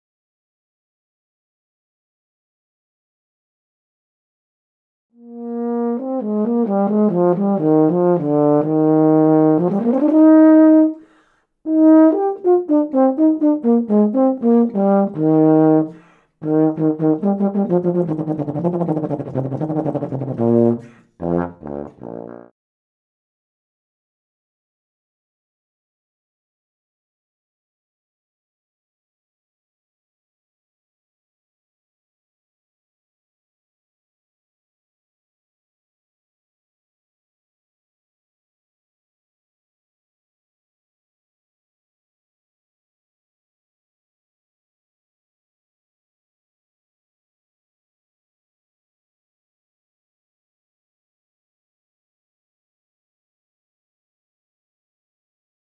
Bombardino en Síb Sound "PremiereII" tocado por Ibai Pastor
Un bombardino Compensado con buena afinación y facilidad de emisión. Este bombardino le permite tocar pasillos rápidos sin problemas y como vea en el vídeo suena bien homogéneo.